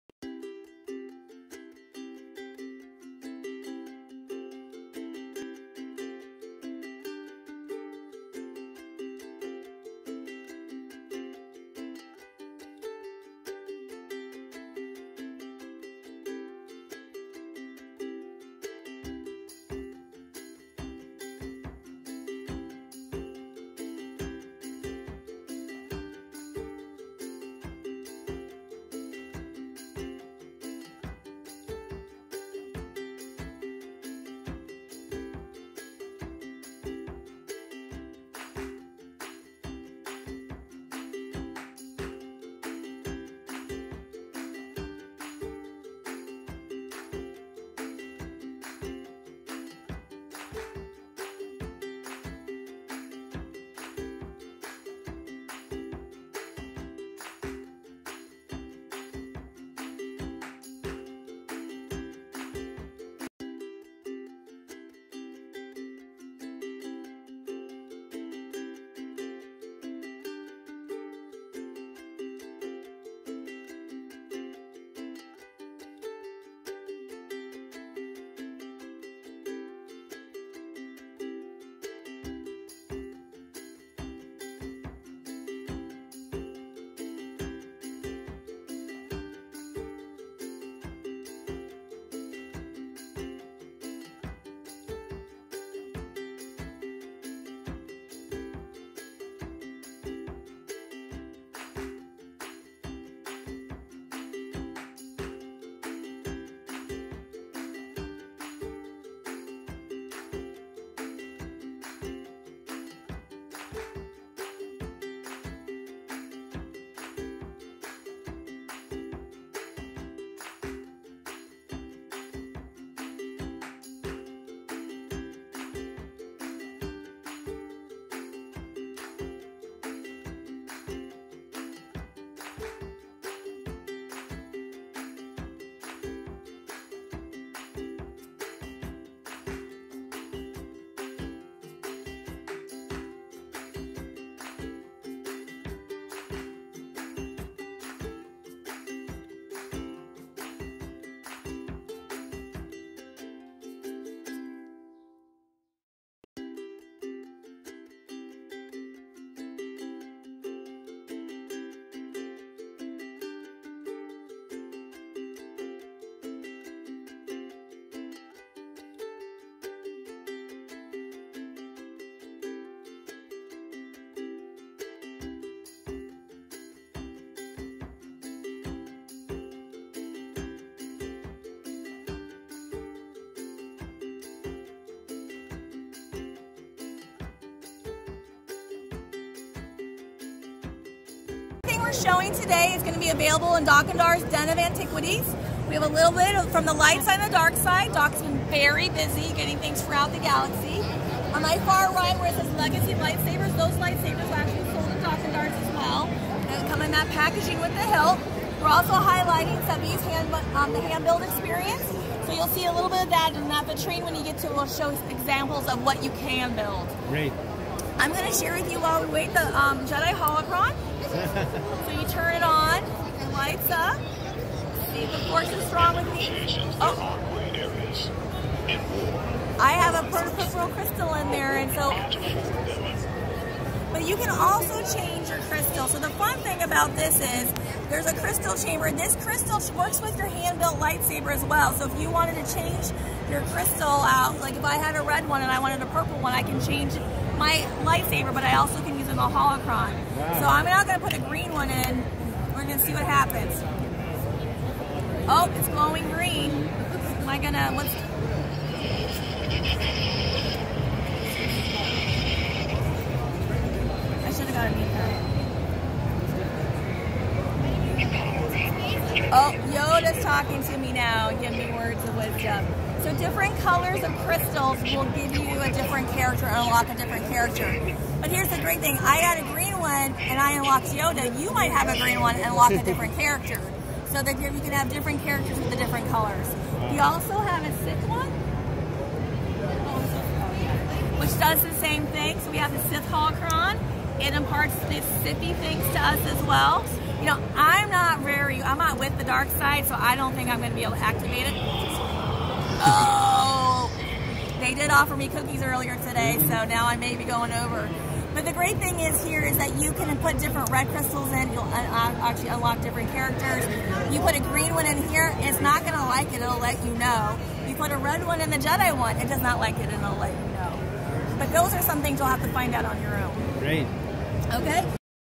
Galaxy's Edge Merchandise Preview
Small preview of the merch that'll be featured at the new parks in both Disneyland and Disney World.  Footage was taken at Star Wars Celebration Chicago.  I missed out on the creatures.

I specialize in Disney, Universal, and all inclusive vacations.